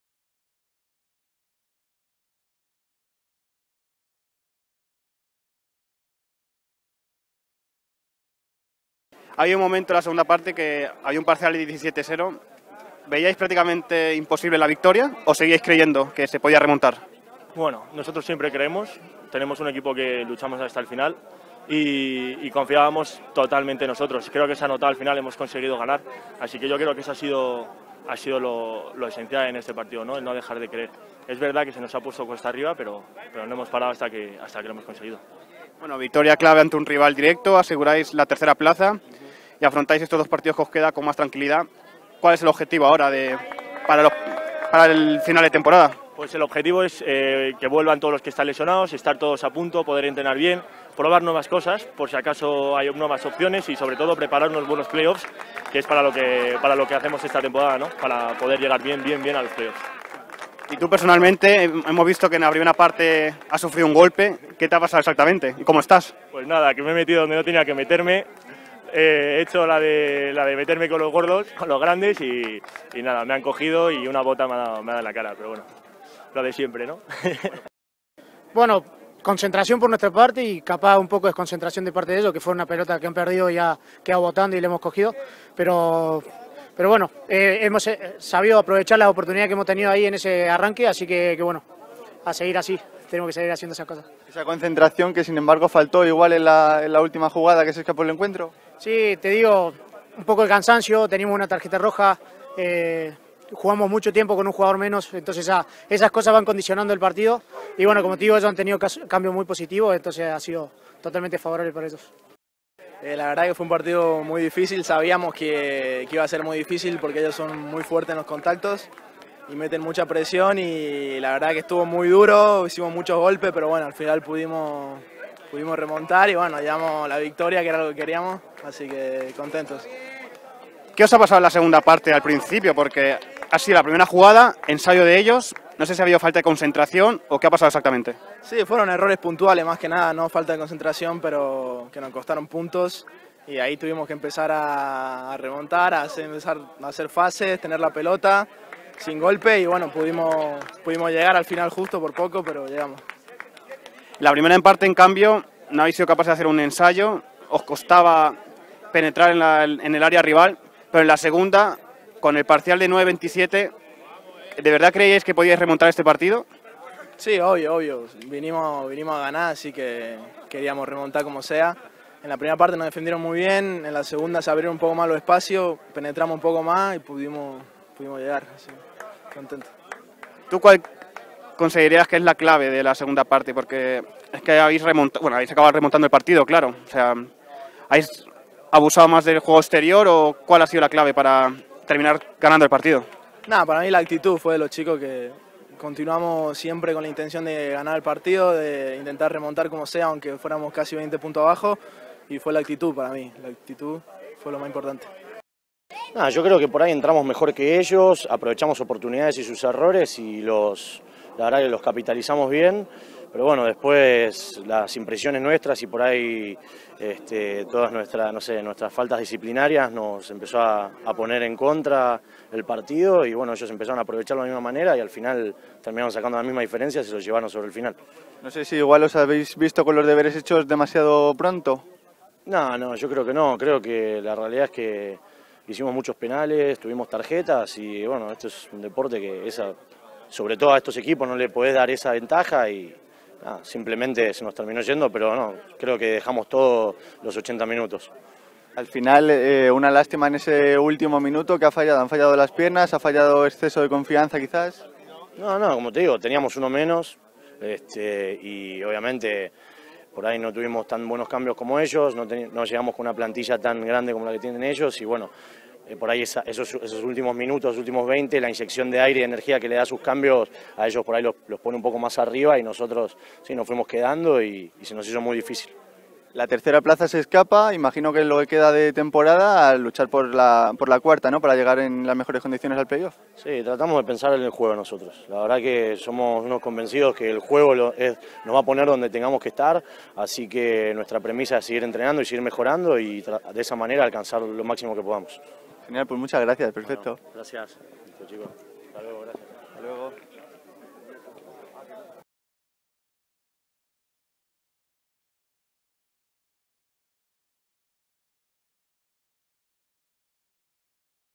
Alcobendas -- Independiente 07-04-2018
Entrevistas con Jaime Mata, Mariano García, Iñaki Mateu, y Juan Pablo Guido tras el Alcobendas -- Independiente (rugby) para